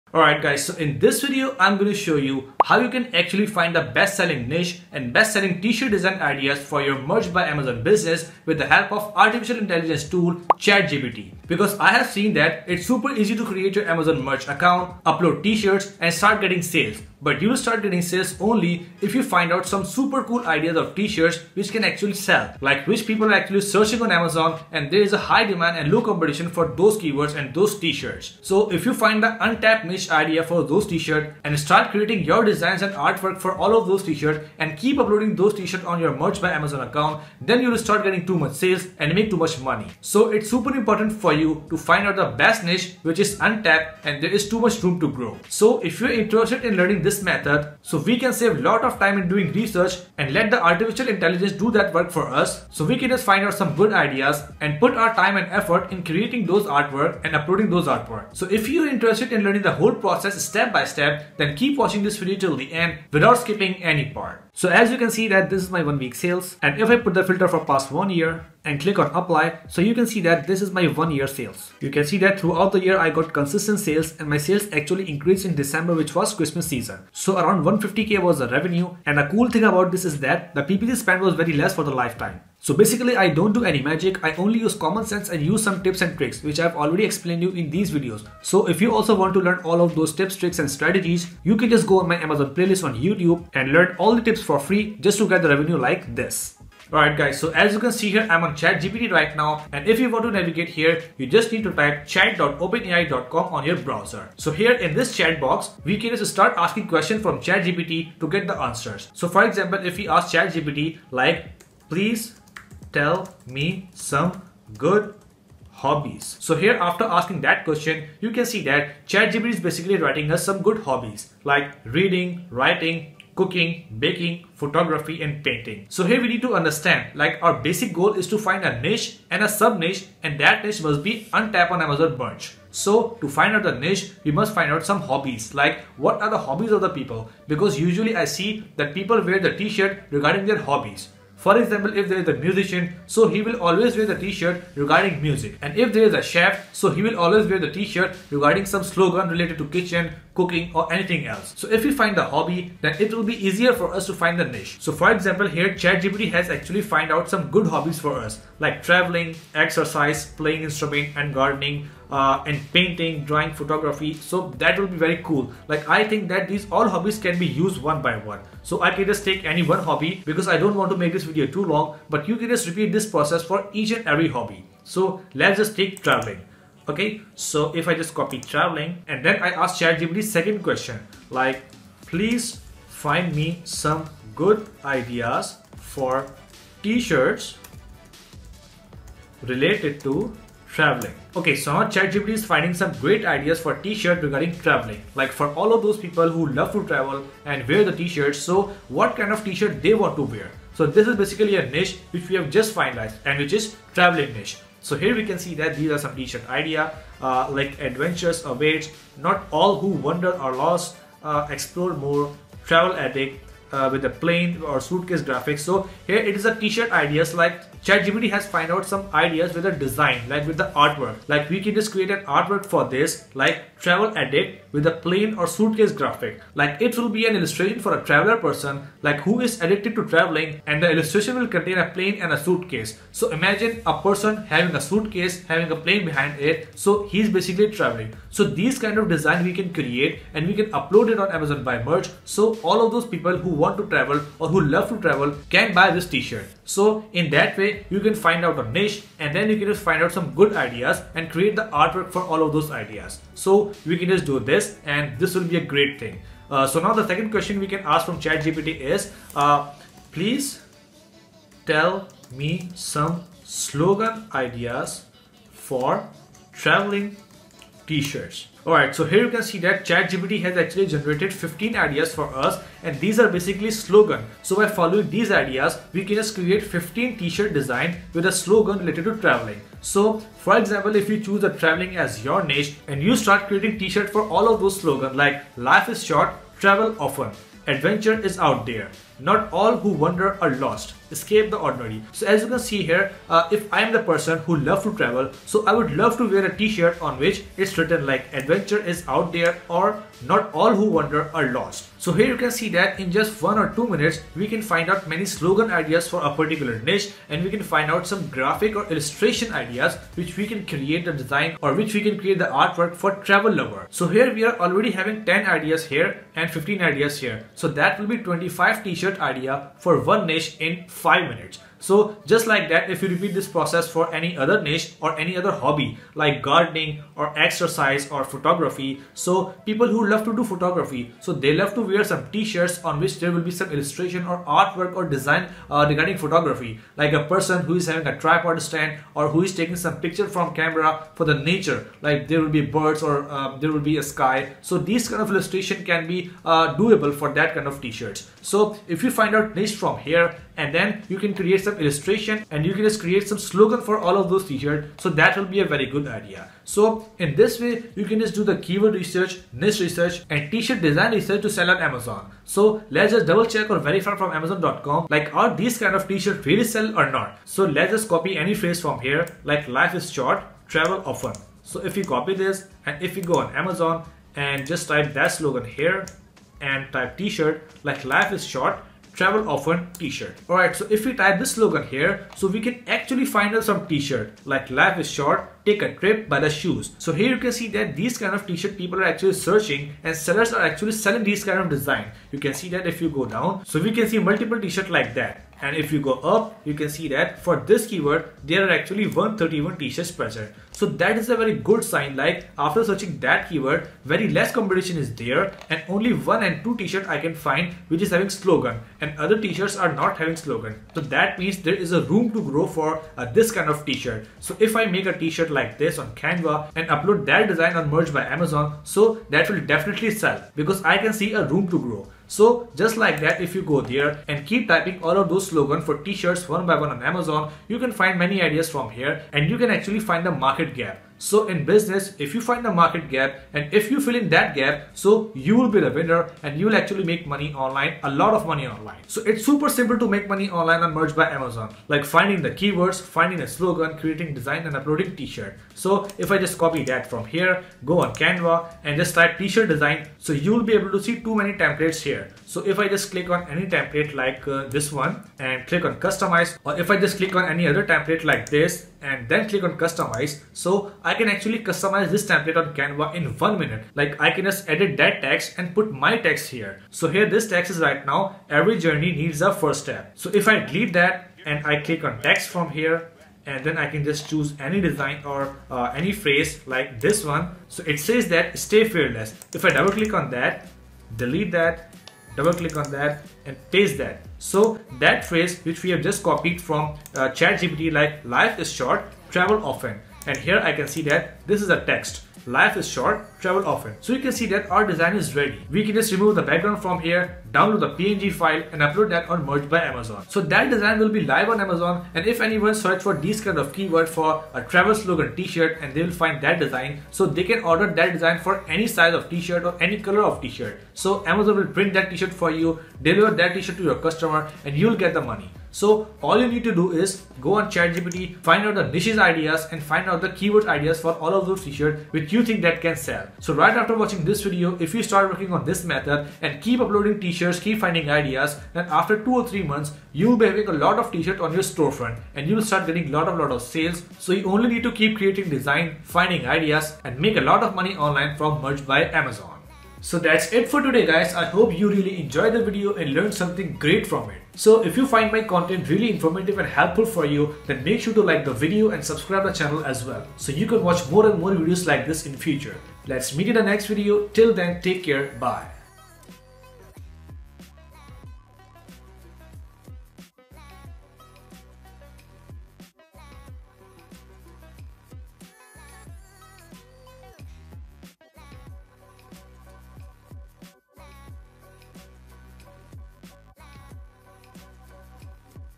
FREE Merch By Amazon Niche Research Using ChatGPT Open AI Artificial Intelligence
If you want to find profitable niche to sell shirts on merch by amazon print on demand then this video is a must watch for you because you will learn how to do profitable merch by amazon niche research using ChatGPT open ai artificial intelligence tool for free and start your amazon print on demand store now and start selling your t shorts on amazon
Keep watching ways to sell online.

✅Checkout The Services We Offer To Grow Your Business

🔥 Checkout The Tools I Use To Grow My Business